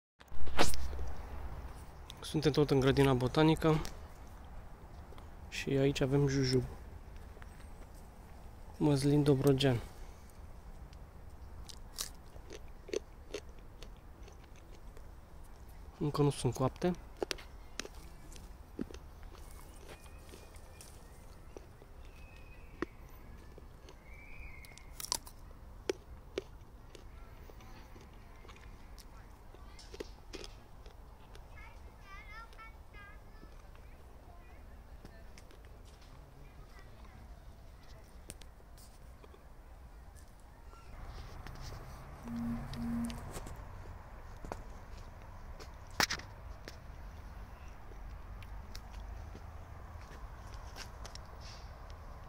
Ziziphus -Jujub - Maslin Dobrogean- Curmal Chinezesc.
Fructele ajung la maturitate deplină în octombrie și persistă după căderea frunzelor, se stafidesc in pom si atunci au cantitatea maxima de zaharuri.
Dupa stafidire au forma si gustul curmalelor.
Sunt pline de vitamine, pomul nu este pretentios si poate fi cultivat si pe solurile saraturoase si acolo unde nu exista irigatii.